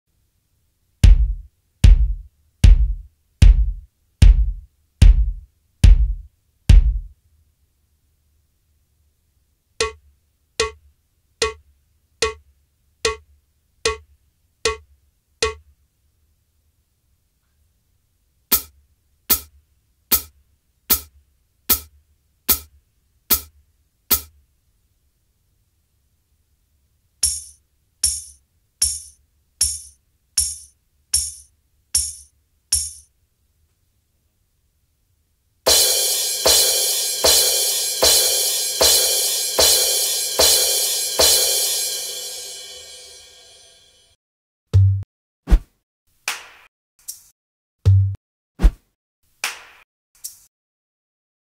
MEINL Percussion Digital Stomp Box - MPDS1 (Detail3)
MEINL Percussion Stomp Box – Digital

Description:
The MEINL Percussion Digital Stomp Box is an easy solution for incorporating a pulse into your music. Choose between five pre-programmed percussion samples, including kick drum, cowbell, hihat, tambourine and crash cymbal - perfect for percussionists, singer/songwriters, and multi-instrumentalists. Use the USB port to import one custom sample of your own!  Plug into any amp or PA with the ¼” input and output jacks, then adjust the volume on the pedal as needed.  The built-in Piezo trigger is velocity sensitive, giving you a seamless dynamic response as you tap your foot at different volumes.  A non-slip bottom keeps the pedal in place and prevents vibration.

Materials:
Solid Mahogany

Features:
Five Digital Pre-programmed Percussion Samples + One Custom Sample
Perfect For Percussionists And Multi-instrumentalists
Solid Mahogany Body
Built-in Piezo Trigger With Volume Control
1/4” Input And Output Jacks

Model Number:
MPSD1